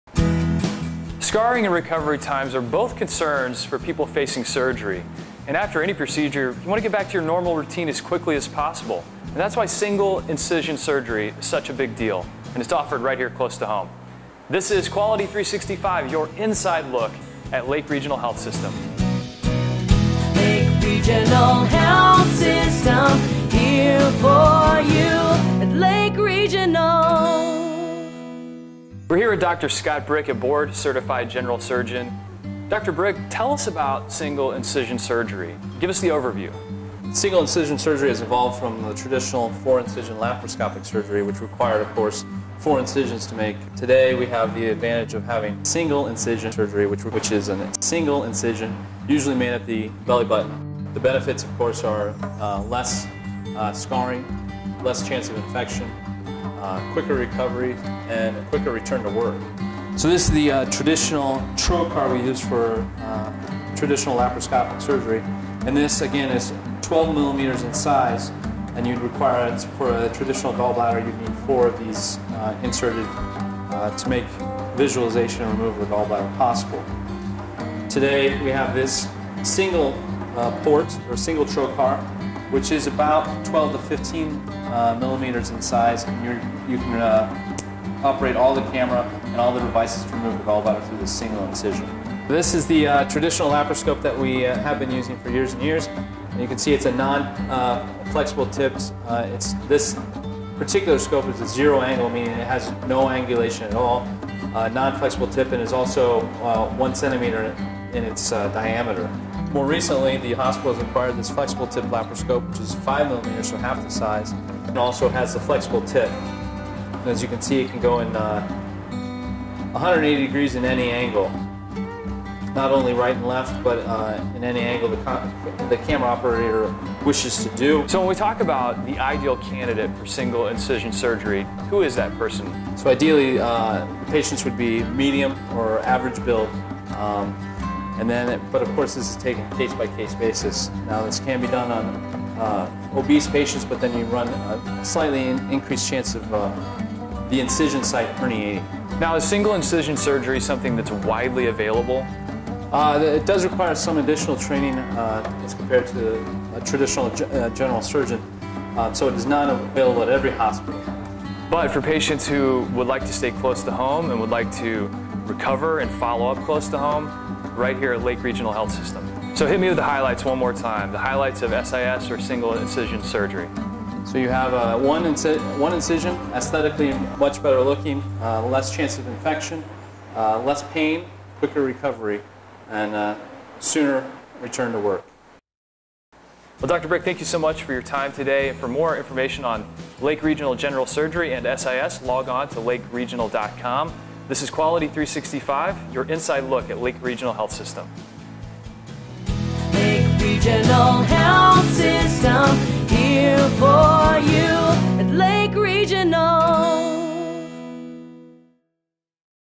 Single Incision Surgery SIS
In this installment we look at the advantages to a newer, minimally invasive surgical technique called single-incision surgery (SIS).  The main advantage to SIS is one small  incision where the belly button is, scarring is minimal.

Other advantages include quicker wound healing, less pain, less chance for infection and quicker recovery time.